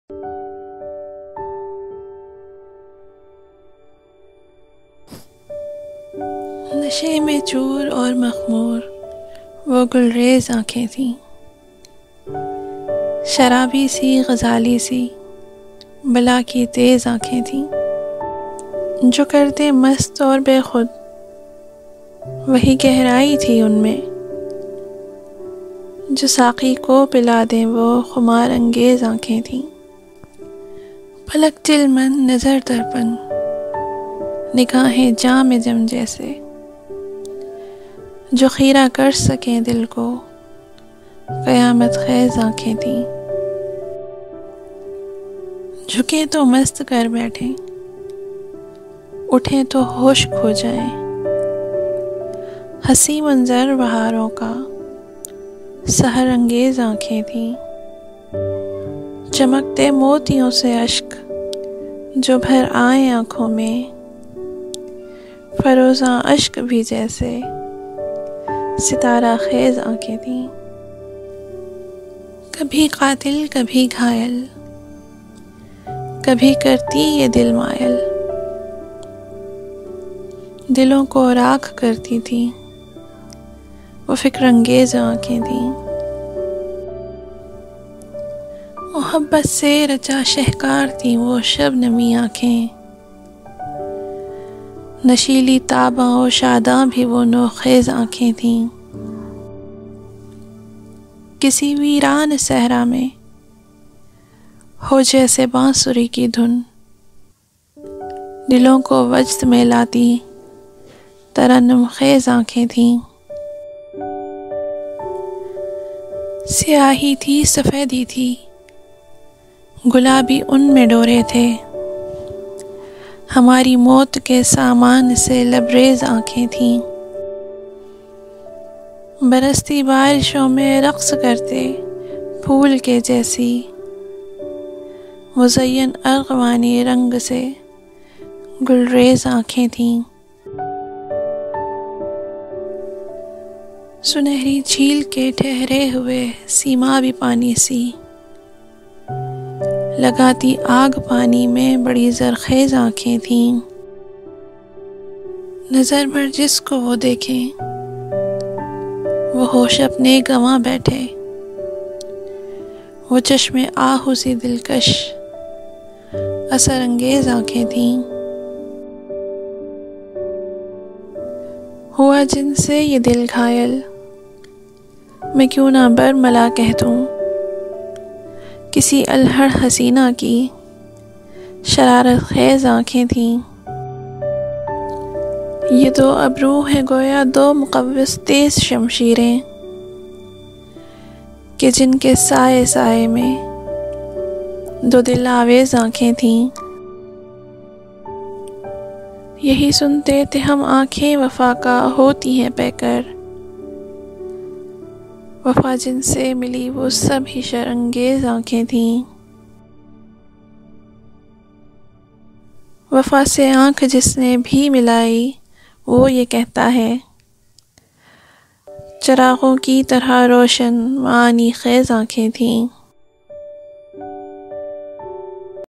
Ankhen - by Wafa Ahmed || Poetry in Urdu / Hindi || Remake of eyes poetry with voice
nashe mein choor aur makhmoor, wo gulrez ankhen thien
sharabi si, ghazali si, bala ki tez ankhen thien

jo kar den mast aur bekhud wohi gehrayi thi un mein
jo saqi ko pila den wo khumaar angez ankhen thien

palak chilman, nazar darpan, nigahen jaam e jim jesi
jo kheera kar saken dil ko qayamat khez ankhen thien

jhuken toh mast kar bethen, uthen toh hosh kho jayen
haseen manzar baharon ka, sahar angez ankhen thien

chamakte motiyon se ashk jo bhar ayen ankhon mein
firozaan ashk bhi jese sitara khez ankhen thien

kabhi qatil kabhi ghayal kabhi kartien yeh dil mayal
dilon ko raakh karti thien wo fikr angez ankhen thien

mohabbat se racha shahkaar thien wo shabnami ankhen
nasheeli taaban o shadaan bhi wo naukhez ankhen thien

kisi veeran sehra mein ho jese baansuri ki dhun
dilon ko wajd mein laati tarannum khez ankhen thien

siyahi thi safedi thi gulabi un mein doray thay
hamari maut ke samaan se labrez ankhen thien

barasti barishon mein raqs karte phool ke jesi
muzayyan arghvani rang se zarkhez ankhen thien

nazar bhar jis ko wo dekhen wo hosh apne ganwa bethe
wo chashm e aahoo si dilkash asar angez ankhen thien

huwa jin se yeh dil ghayal mein kyun na barmala keh doon
kisi alhad haseena ki shararat khez ankhen thien

yeh do abroo hain goya do muqawwas tez shamsheeren
ke jin ke saye saye mein do dil aawez ankhen thien

yehi sunte thay hum ankhen wafa ka hoti hain paikar
wafa jin se mili woh sab hi shar angez ankhen thien

wafa se ankh jis ne bhi milai woh yeh kehta hai
chiraaghon ki tarah roshan, ma'ani khez ankhen thien

Wafa Ahmed